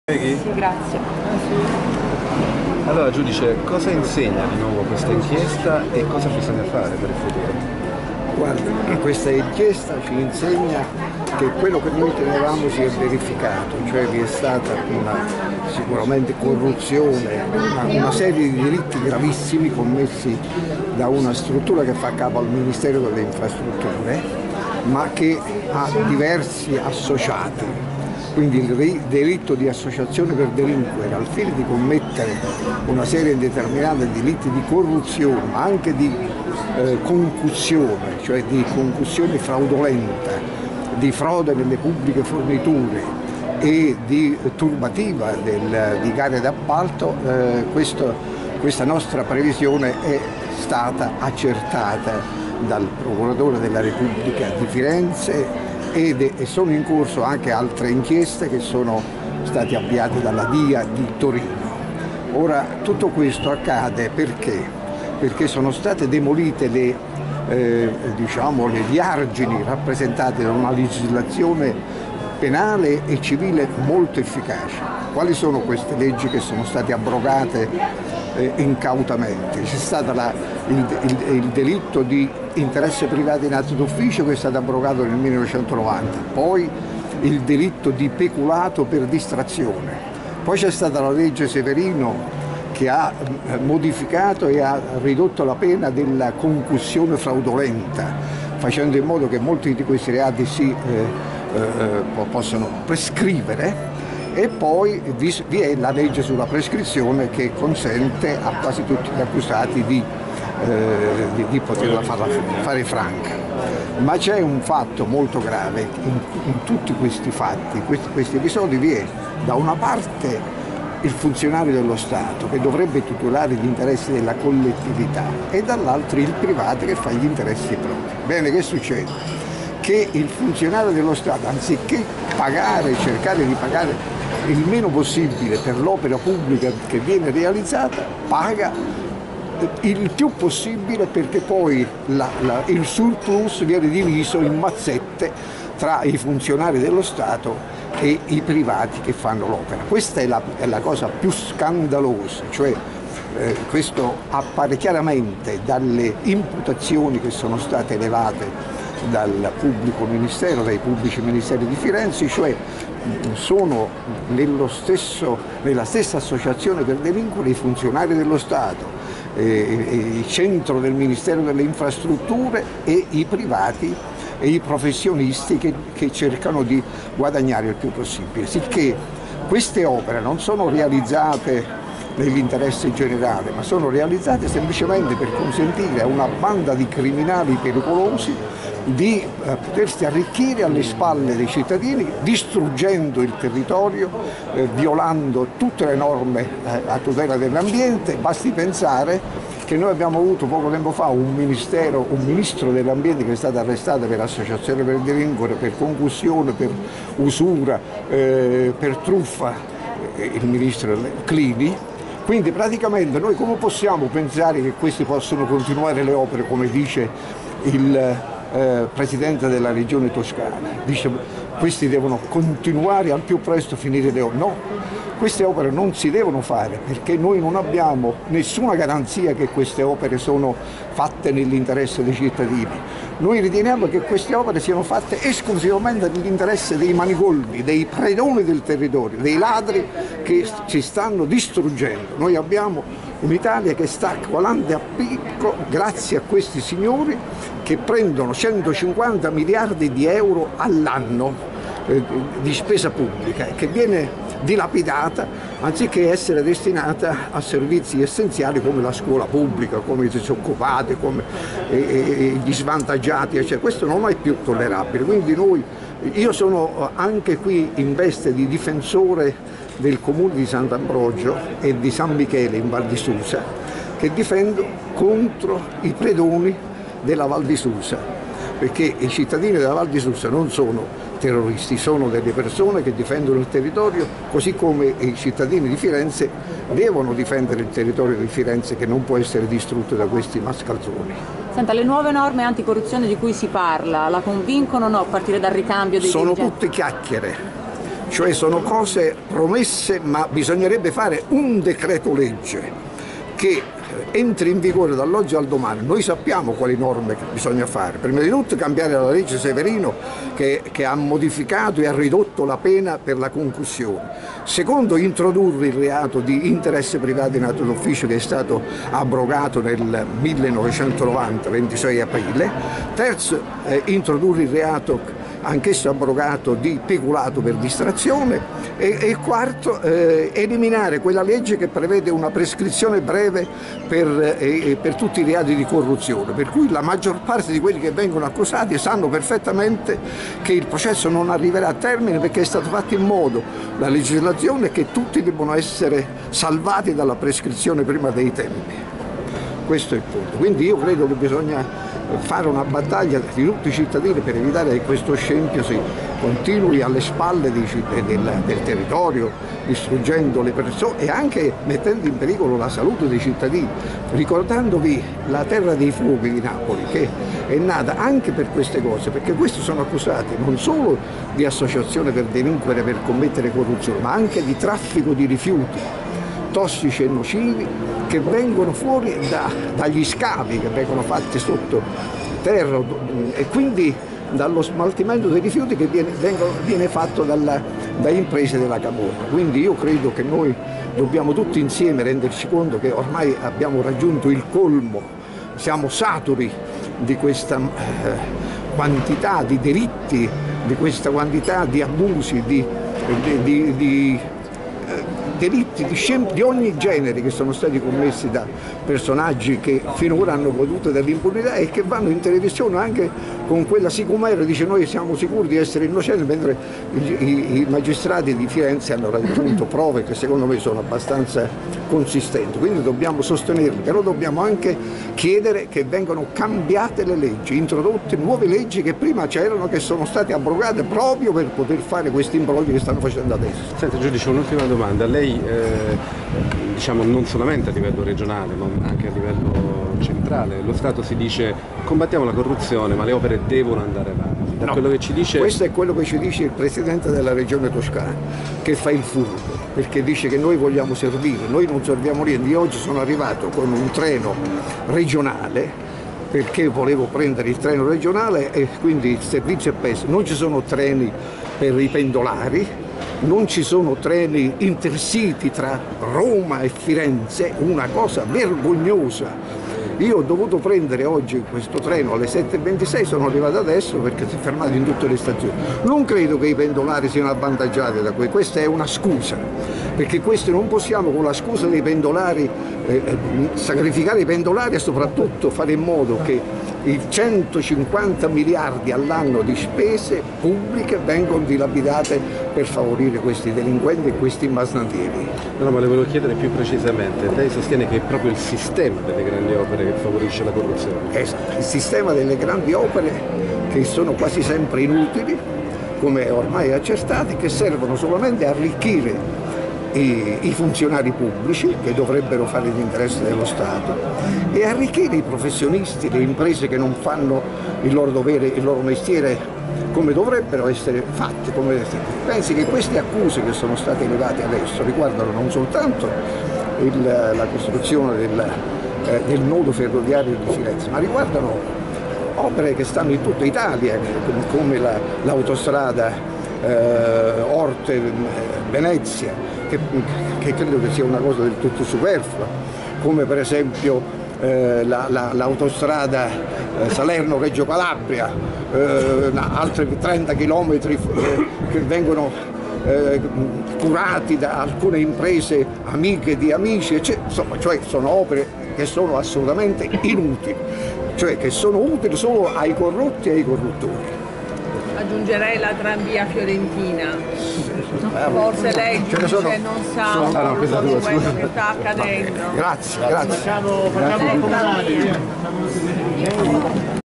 INCHIESTA-SISTEMA, TAV, ANTICORRUZIONE: NUOVI SCENARI?, 2 - F. Imposimato, 1
INCHIESTA-SISTEMA, TAV, ANTICORRUZIONE: NUOVI SCENARI?
Conferenza stampa dell’Associazione Idra con Ferdinando Imposimato, Massimo Bongiovanni e Girolamo Dell’Olio
Firenze, Gran Caffè “Giubbe Rosse”, 23 marzo 2015